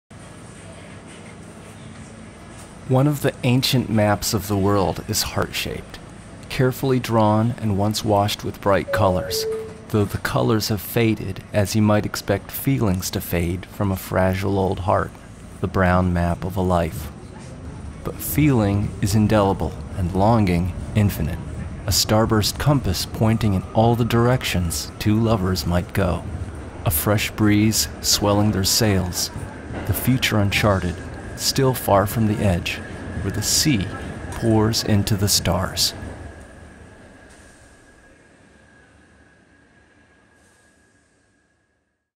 SUBWAY POETRY: A Map of the World
The fabulous Maria Schneider had written some music based on some of Ted Kooser's poems, so I was happy to see some more of his stuff on the train.